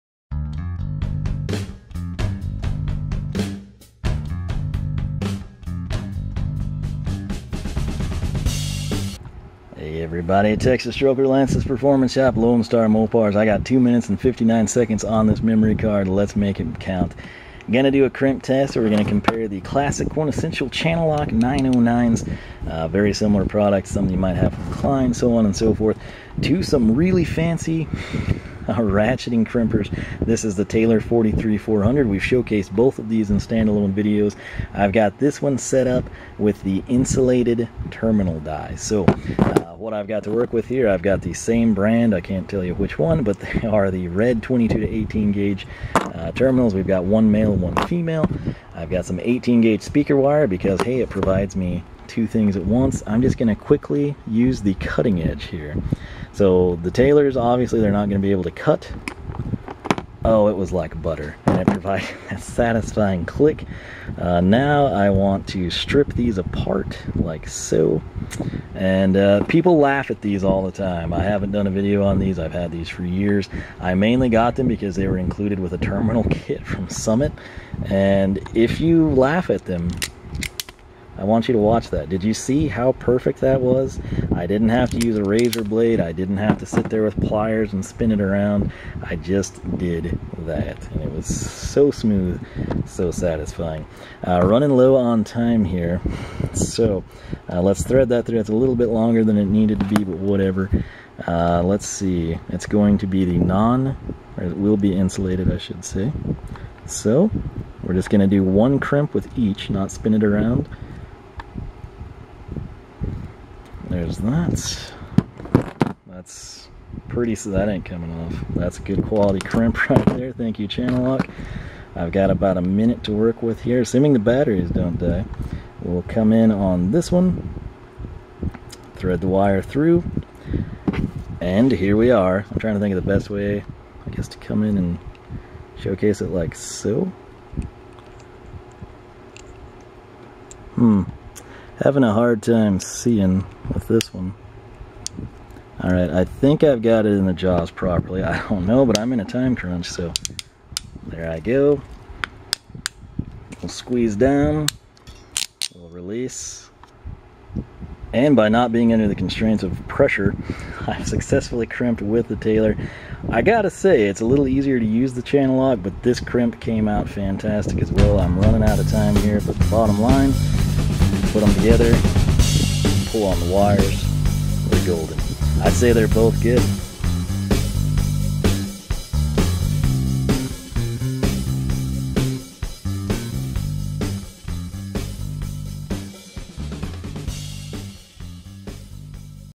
Crimp Test Showdown Channellock 909 vs Taylor 43400
In this video we pit our new crimpers in a head-to-head showdown...which one crimps better...quicker?  Can I get both crimps done before we max out the SD Card?

Which one do you prefer?  I have a lot of experience with 909s and can quickly fire them off...this was my first time to ever crimp with the Taylor 43400.  Moving forward, I will likely use both regularly.